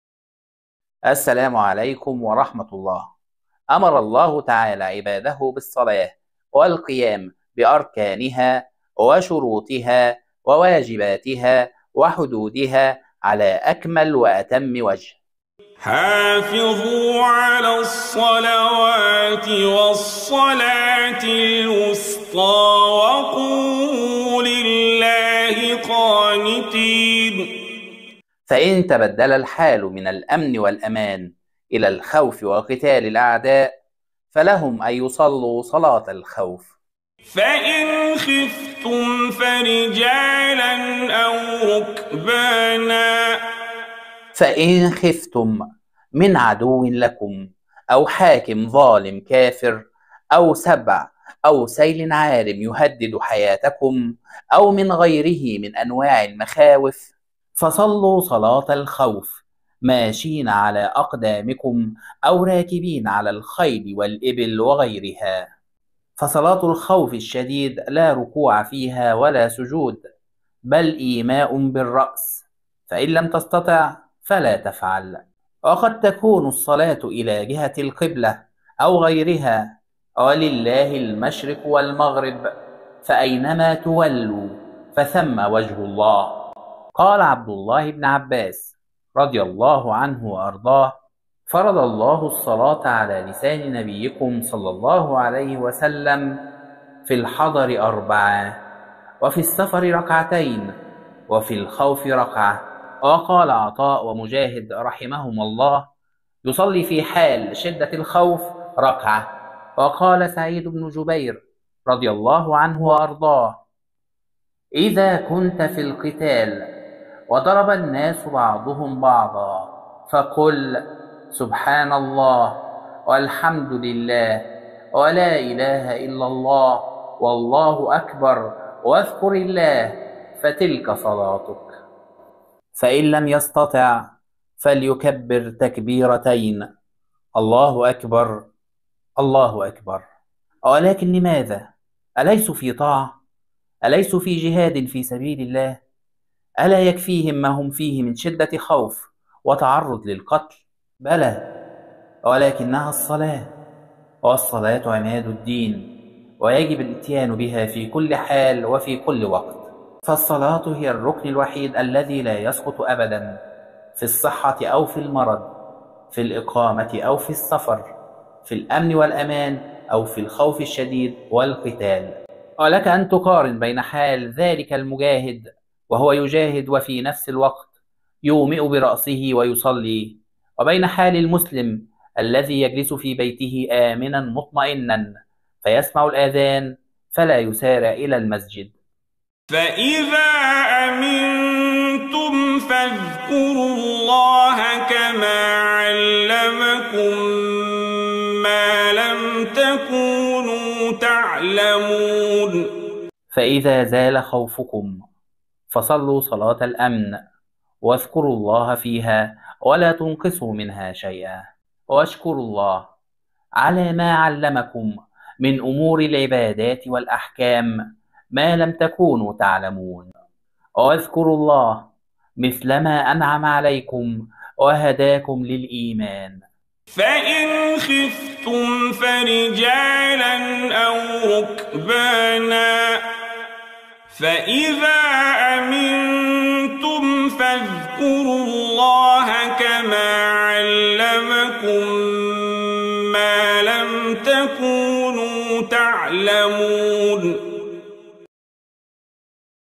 تفسير البقرة ٢٣٩: فإن خفتم فرجالا أو ركبانا ۖ فإذا أمنتم فاذكروا الله كما علمكم ما لم تكونوا تعلمون
فَإِنْ خِفْتُمْ فَرِجَالًا أَوْ رُكْبَانًا ۖ فَإِذَآ أَمِنتُمْ فَٱذْكُرُواْ ٱللَّهَ كَمَا عَلَّمَكُم مَّا لَمْ تَكُونُواْ تَعْلَمُونَ

التفسير الميسر : فإن خفتم من عدو لكم فصلوا صلاة الخوف ماشين، أو راكبين، على أي هيئة تستطيعونها ولو بالإيماء، أو إلى غير جهة القبلة، فإذا زال خوفكم فصلُّوا صلاة الأمن، واذكروا الله فيها، ولا تنقصوها عن هيئتها الأصلية، واشكروا له على ما علَّمكم من أمور العبادات والأحكام ما لم تكونوا على علم به.